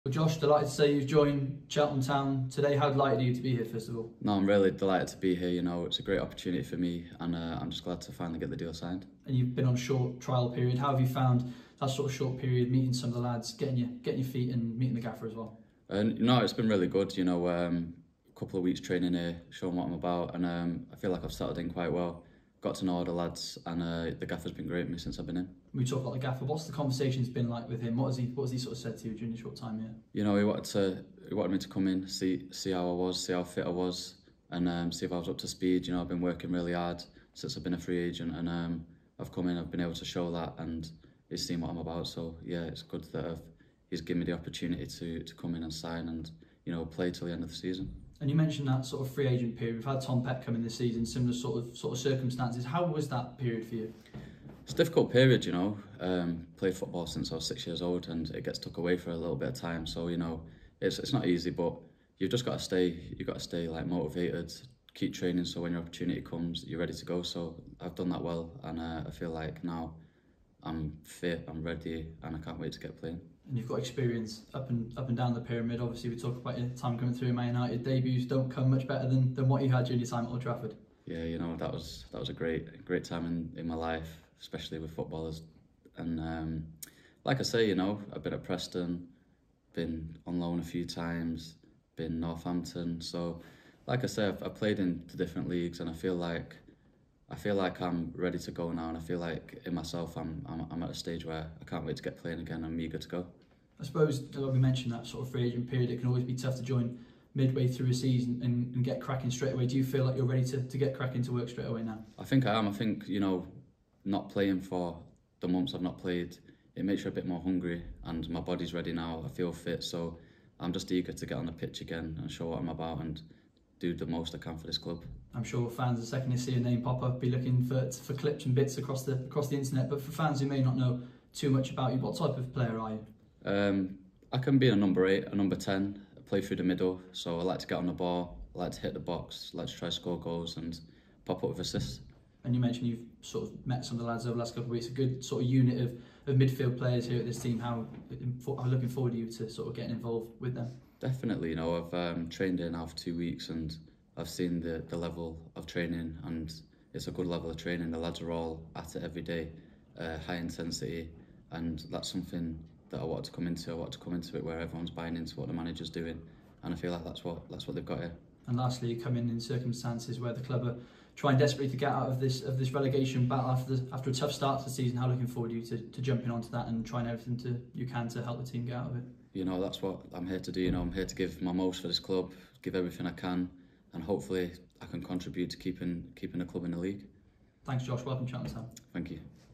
Welcome to Town Josh Harrop
We sat down with new Robins midfielder Josh Harrop after he became the club's first signing of the January transfer window.